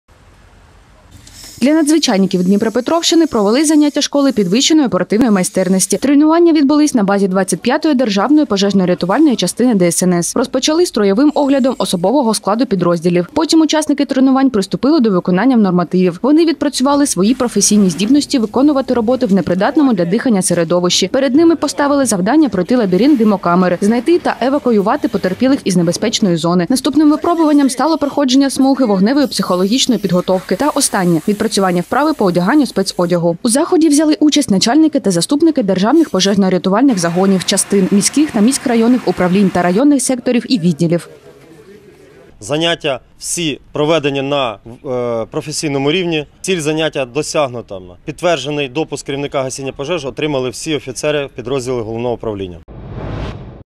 Надзвичайники підвищували професійну майстерність
Для надзвичайників Дніпропетровщини провели заняття школи підвищеної оперативної майстерності. Тренування відбулись на базі 25 державної пожежно-рятувальної частини ДСНС.